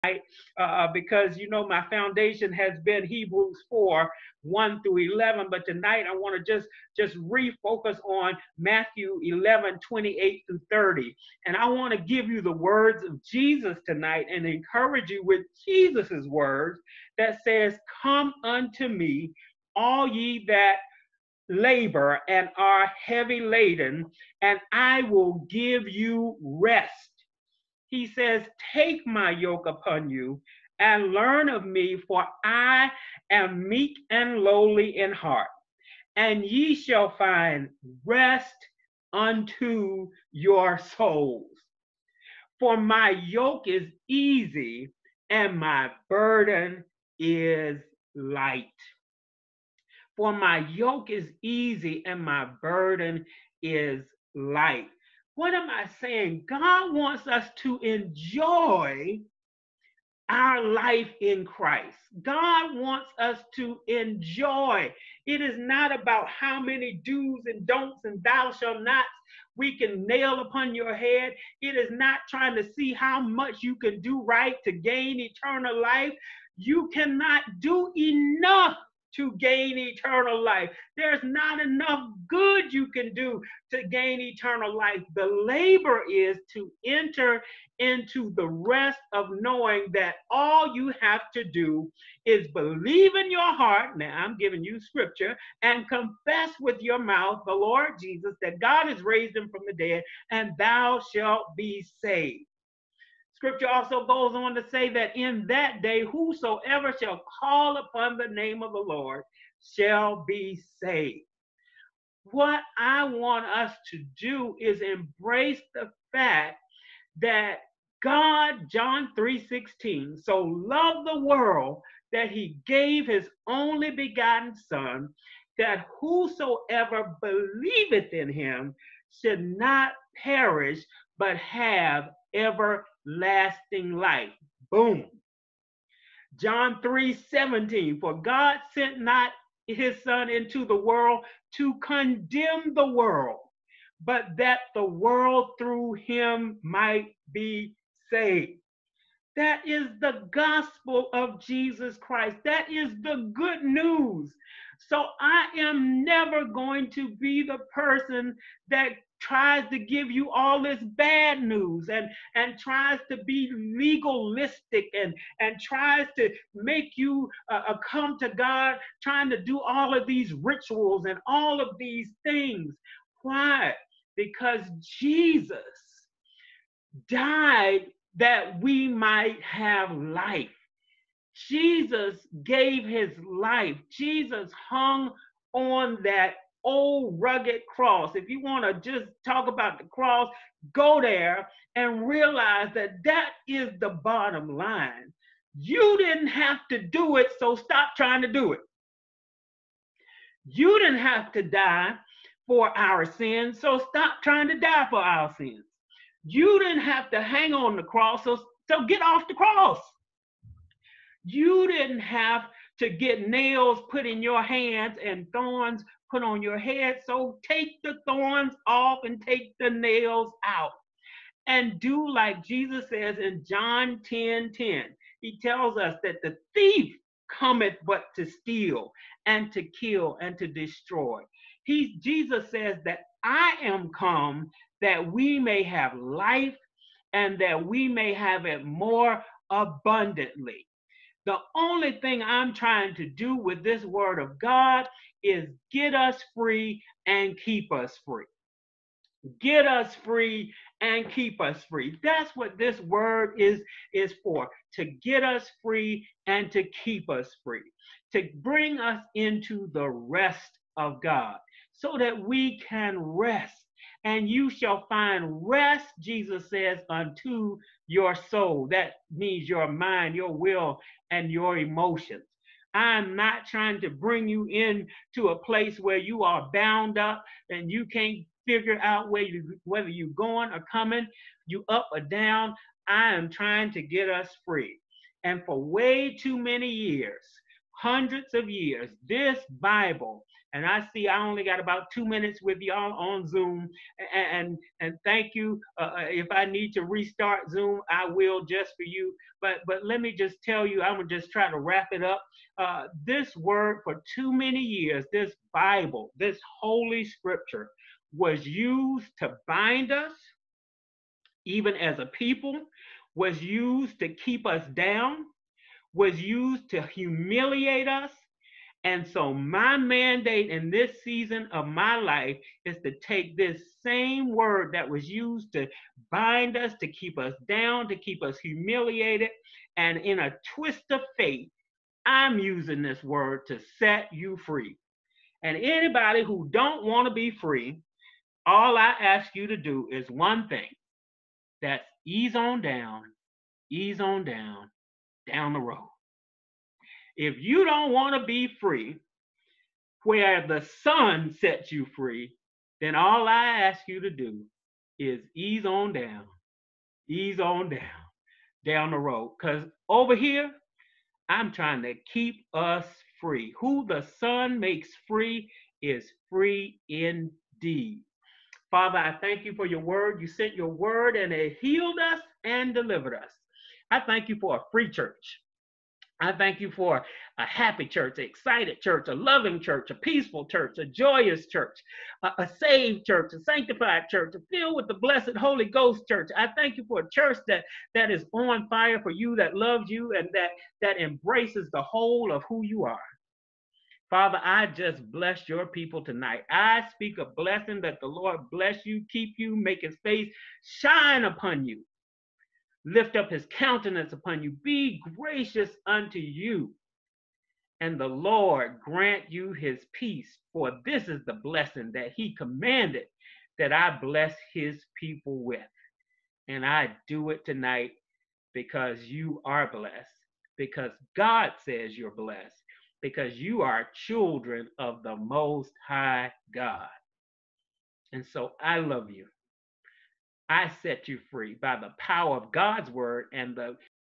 Living Life Light and Easy
Global Harvest Fellowship "The Thinking Church"
Bishop and Pastor James "Rudy" Brown
High Point, NC

Text: Matthew 11:28-30